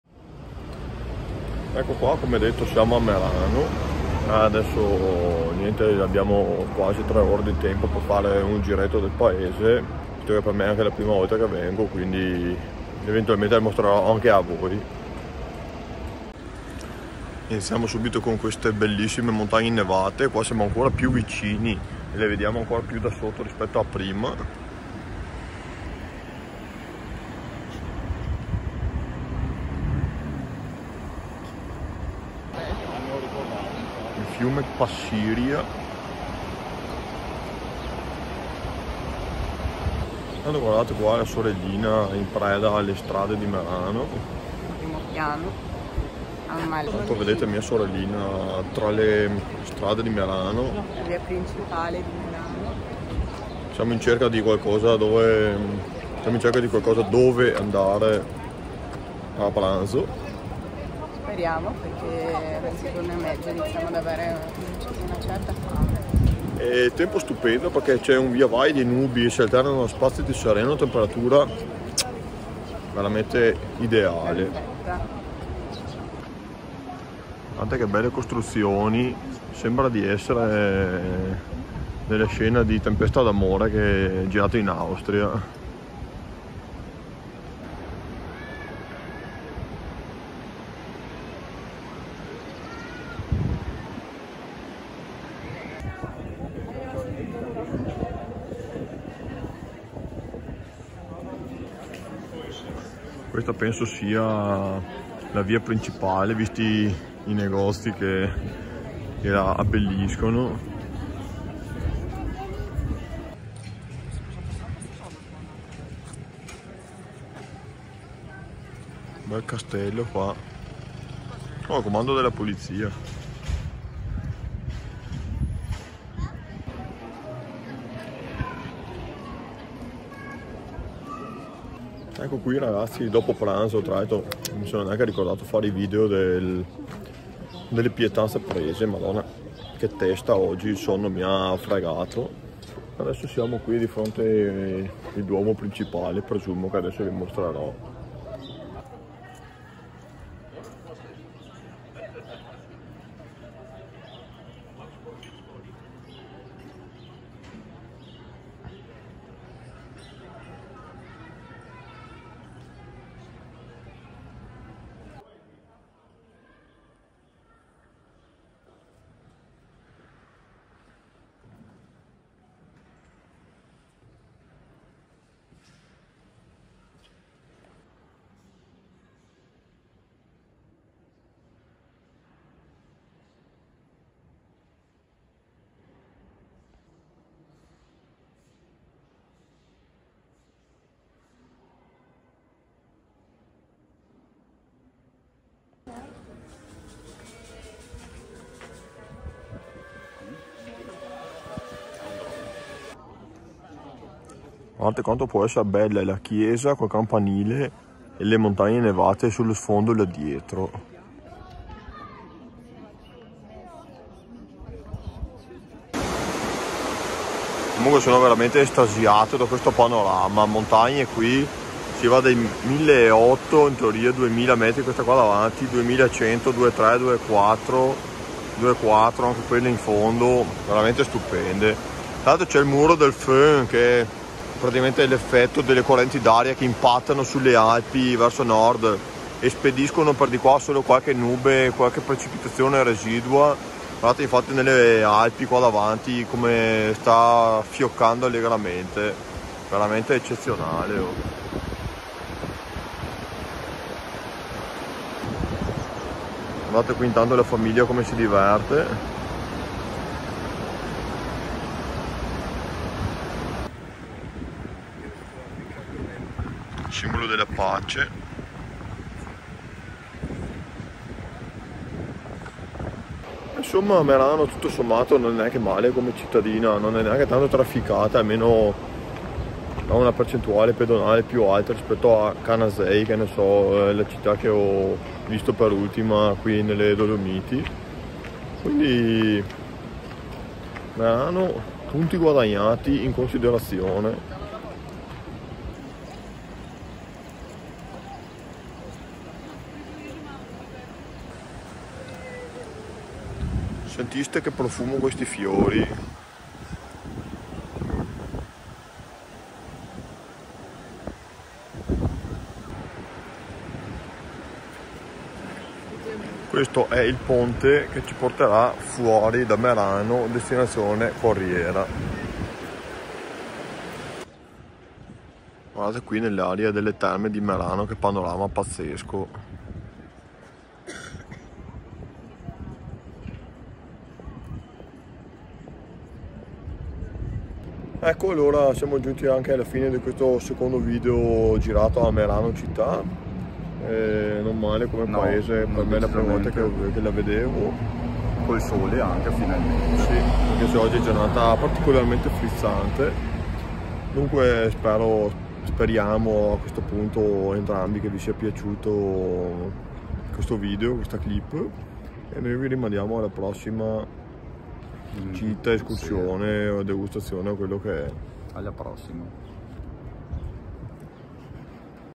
Vi porto a fare due passi a MERANO - Trentino Alto Adige
Ciao a tutti ragazzi e bentornati!

Oggi breve video direttamente dalla cittadina di Merano, paese dell’Alto Adige posto tra la Val Passiria, la Val Venosta e la Val d’Adige.

6 minuti di video leggerissimi e piacevolissimi, dove in compagnia di mio fratello e di mia sorella vi portiamo tra le vie e sul lungofiume di questa cittadina carina incastonata tra le montagne.

Buona visione!